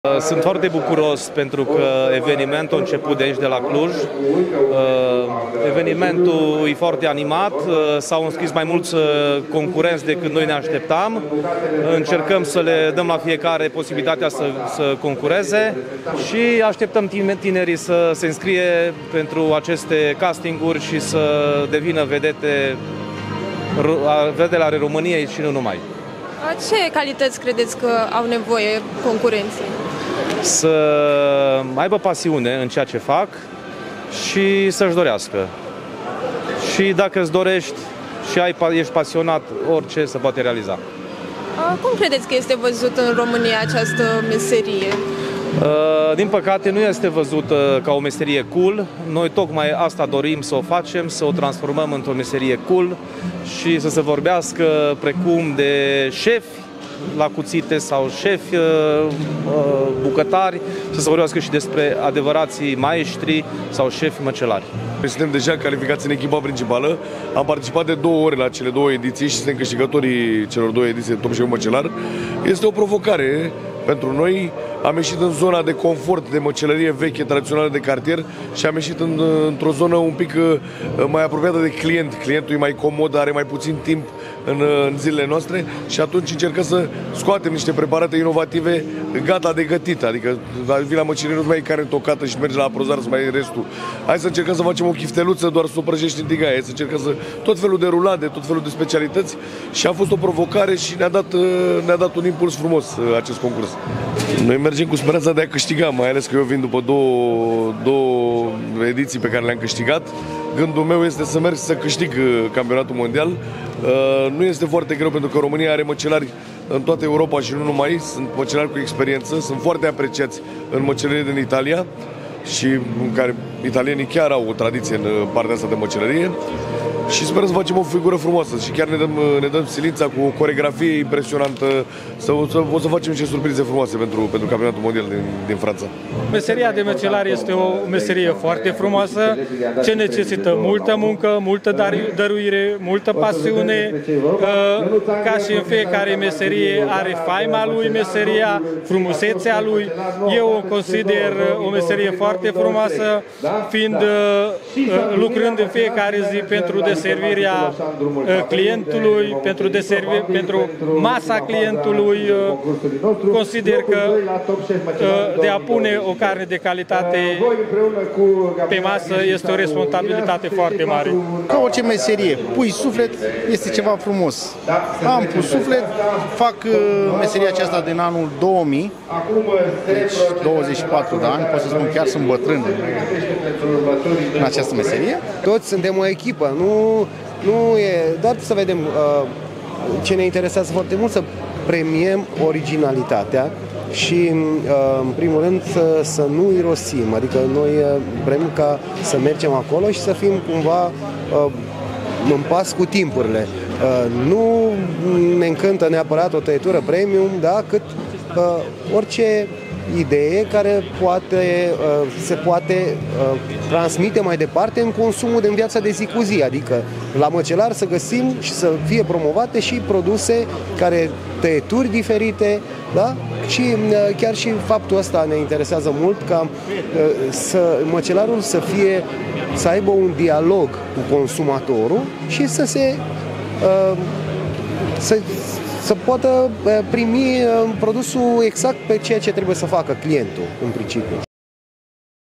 Selecția „Măcelari de elită” la Cluj-Napoca 2024
Se caută „Măcelari de elită” pentru echipa care va reprezenta România la World Butchers’ Challenge. Zece locuri sunt disponibile în echipa care va merge la cel mai prestigios eveniment mondial dedicat măcelăriei, programat anul viitor, la Paris. România este singura țară din sud-estul Europei care va concura în această competiție.